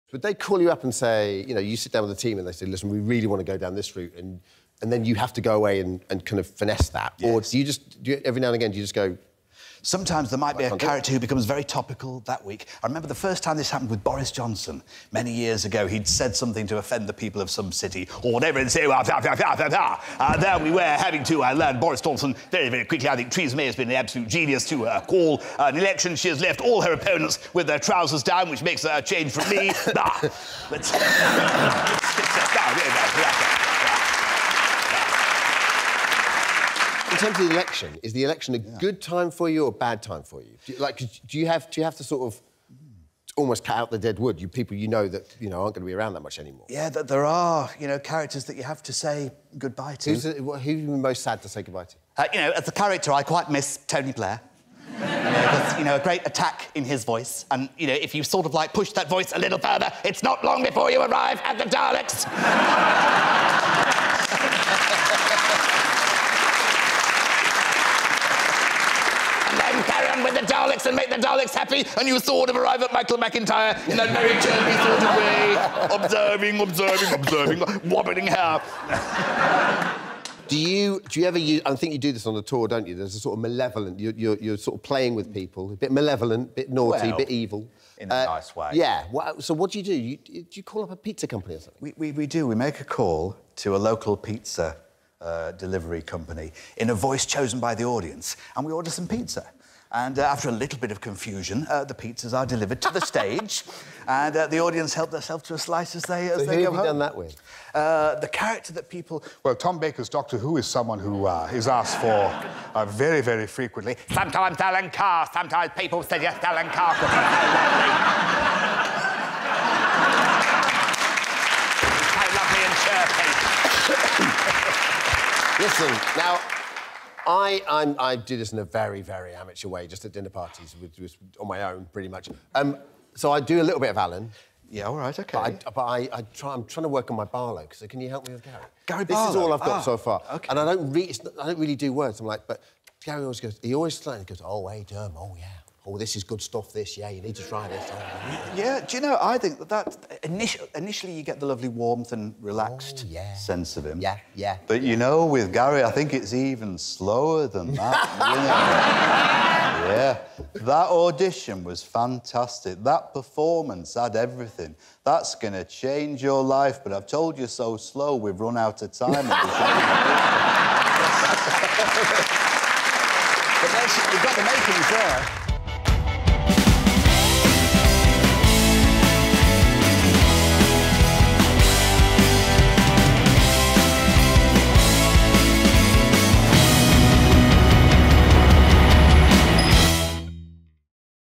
Jon Culshaw's Brilliant Political Impressions
Tony Blair, Boris Johnson.. the list is endless.